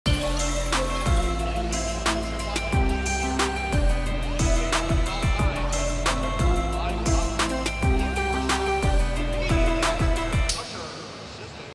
There's a SNOW storm brewing in Raleigh! ❄️❄️❄️
Jalen Chatfield makes sure the bench is nice and awake for some Game 2 warmups!

Feeling social?